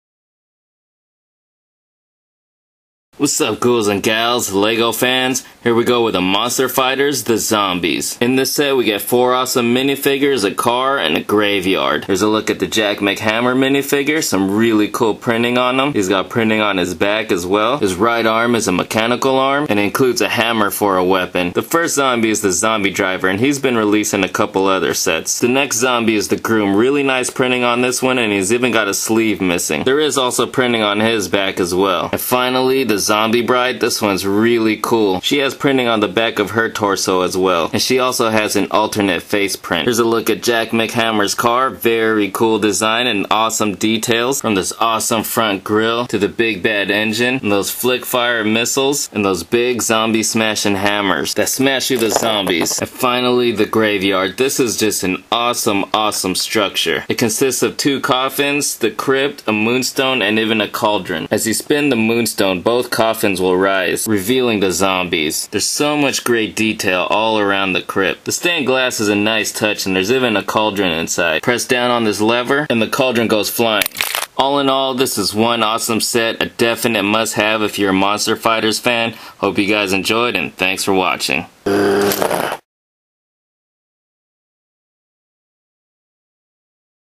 LEGO Monster Fighters 9465 The Zombies Quick Review
One of the best Monster Fighters set! Retails for $39.99.